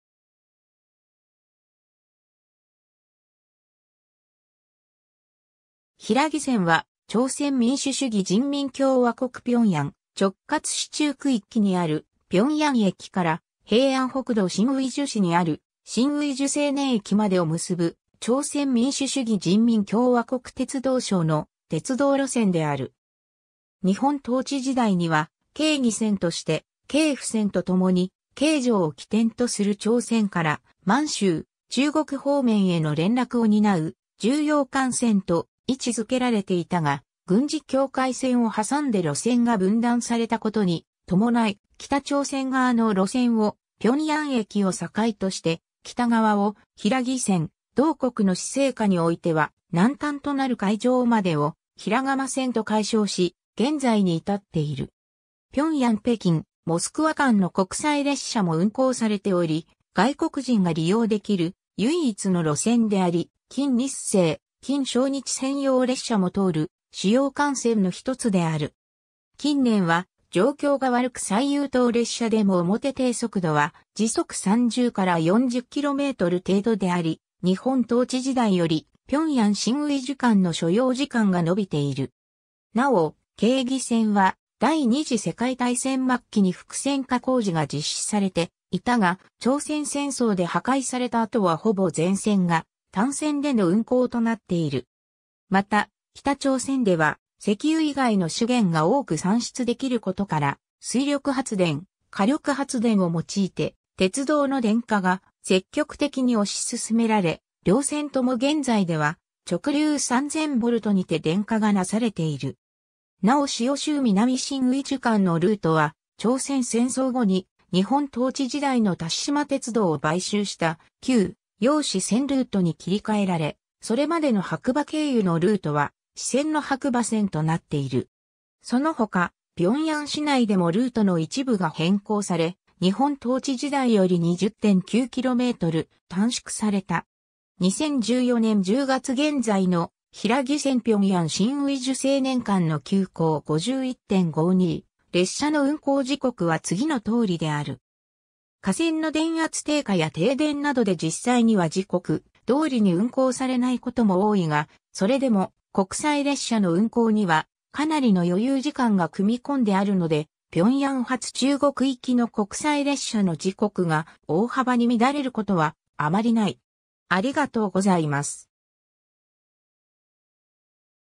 平義線
平義線（ピョンイせん）は、朝鮮民主主義人民共和国平壌直轄市中区域にある平壌駅から平安北道新義州市にある新義州青年駅までを結ぶ、朝鮮民主主義人民共和国鉄道省の鉄道路線である。
 日本統治時代には京義線として、京釜線と共に京城（現：ソウル）を起点とする朝鮮から満州・中国方面への連絡を担う重要幹線と位置づけられていたが、軍事境界線を挟んで路線が分断されたことに伴い、北朝鮮側の路線を平壌駅を境として北側を平義線、同国の施政下においては南端となる開城までを平釜線（釜は釜山を指す）と改称し、現在に至っている。
 平壌-北京・モスクワ間の国際列車も運行されており、外国人が利用できる唯一の路線であり、金日成・金正日専用列車も通る主要幹線の一つである。
近年は状況が悪く最優等列車でも表定速度は時速30-40km程度であり、日本統治時代より平壌-新義州間の所要時間が伸びている。
 なお、京義線は第二次世界大戦末期に複線化工事が実施されていたが、朝鮮戦争で破壊された後はほぼ全線が単線での運行となっている。
また北朝鮮では石油以外の資源が多く産出できることから、水力発電・火力発電（石炭）を用いて鉄道の電化が積極的に推し進められ、両線とも現在では直流3000Vにて電化がなされている。
 なお塩州～南新義州間のルートは、朝鮮戦争後に日本統治時代の多獅島鉄道を買収した旧：楊市線ルート（龍川経由）に切り替えられ、それまでの白馬経由のルートは支線の白馬線となっている。
そのほか、平壌市内でもルートの一部が変更され、日本統治時代より20.9km短縮された。
 2014年10月現在の、平義線平壌 - 新義州青年間の急行51・52列車の運行時刻は次の通りである。
架線の電圧低下や停電などで実際には時刻通りに運行されないことも多いが、それでも国際列車の運行にはかなりの余裕時間が組み込んであるので、平壌発中国行きの国際列車の時刻が大幅に乱れることはあまりない。